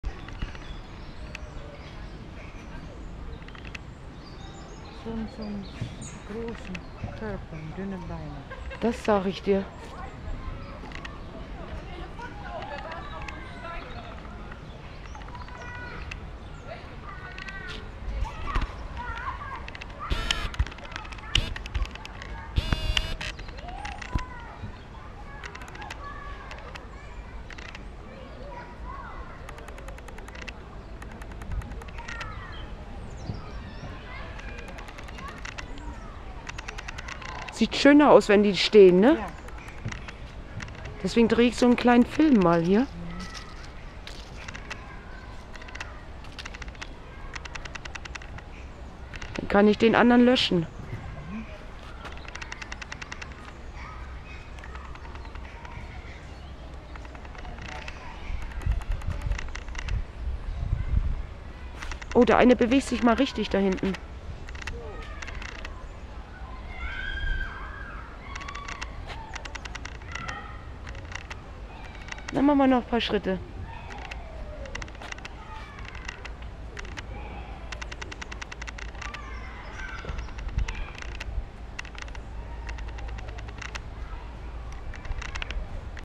Sparziergang durch den Tierpark Nordhorn Teil :10 , Dienstag den 27.09.2011
Das sind ganz schöne große Tiere die da in der Herde sind.